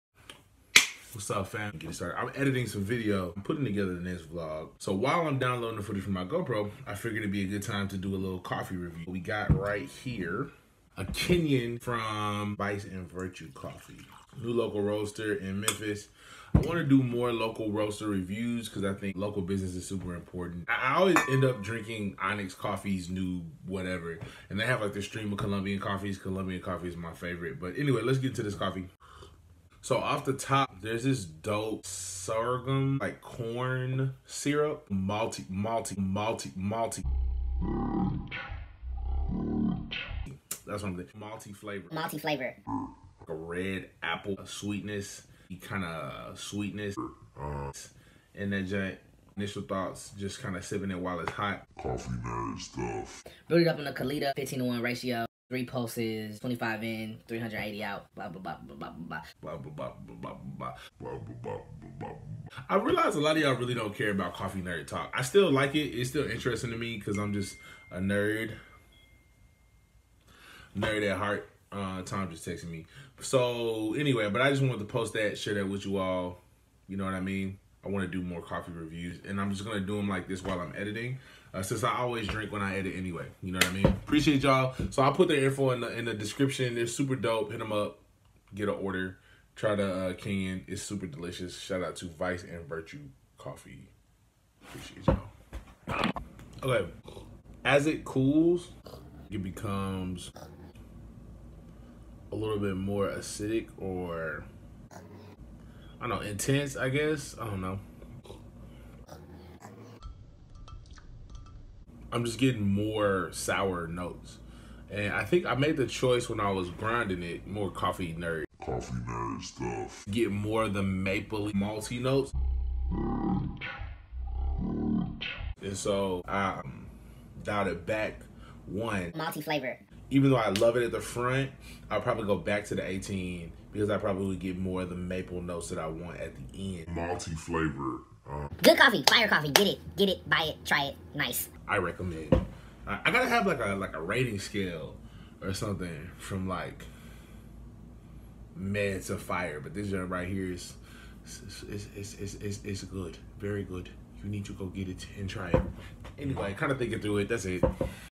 Black Dad Reviews Local Coffee Roaster | Vice & Virtue Coffee Review
In his episode I drink too much coffee and talk about it in a very nerdy way. Enjoy!